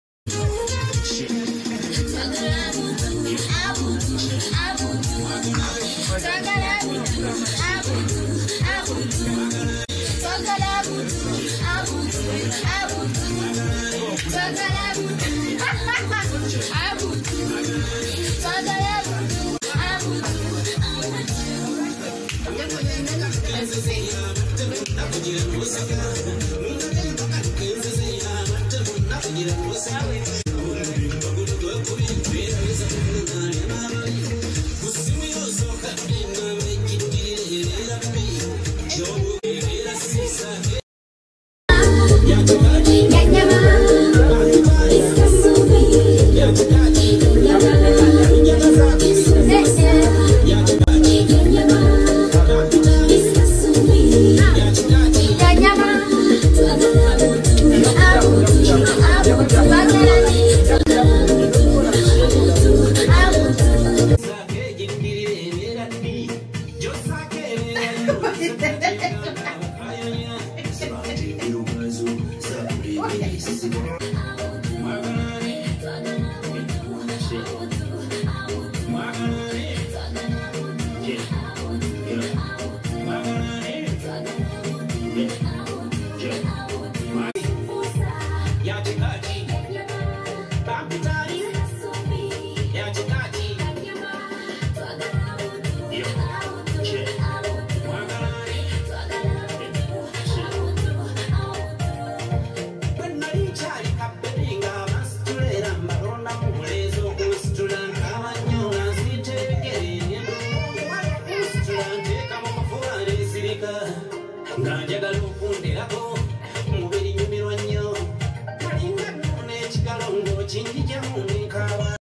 BATABUSE LWA ABDUL MULAASI BEYAMBUDE ENGOYE
Abana bawala abato bamukuba kyeyo bongede okulumya abakade abakayanila abdu mulaasi